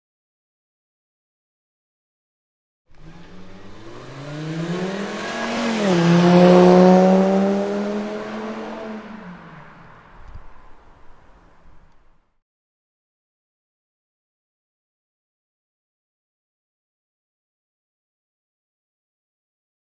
Volkswagen Rabbit - MagnaFlow Exhaust Part #16692
Drive by of a Volkswagen Rabbit with a MagnaFlow Exhaust Part #16692